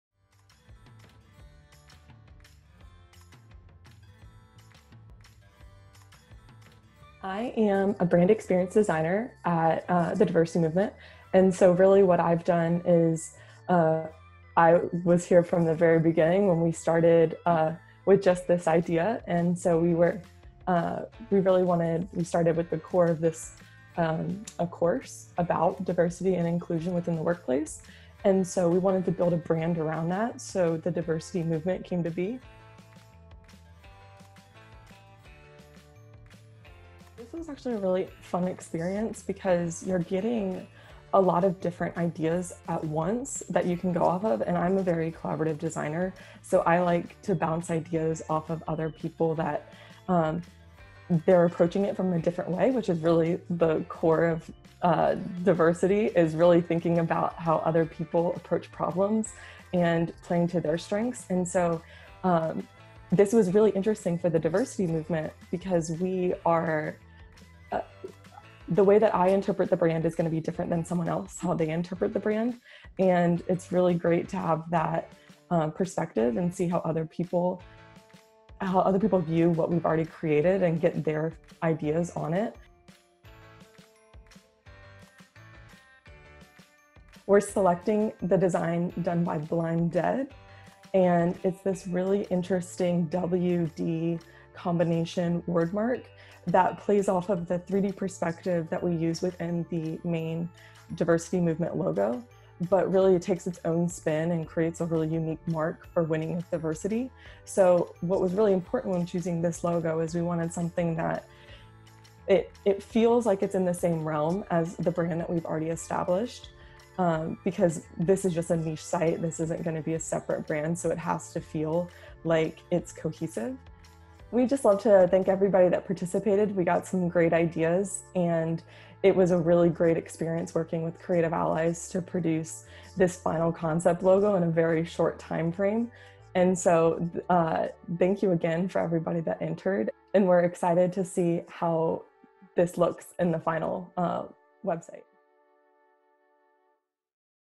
Logo Design Competition Winner Announcement: Winning With Diversity
The Diversity Movement was looking for a new logo for one of its initiatives, Winning With Diversity. They came to Creative Allies for help. We hosted a graphic design competition to find the perfect logo and get great fan engagement for the brand. This logo design competition was a great opportunity for designers to create something special for a new startup focused on DEI.

In this video, hear from Amanda Bennett as she talks about her experience with the logo design contest and how she chose the winning design.

Congratulations to Blind Dead, the Grand Prize Winner!!!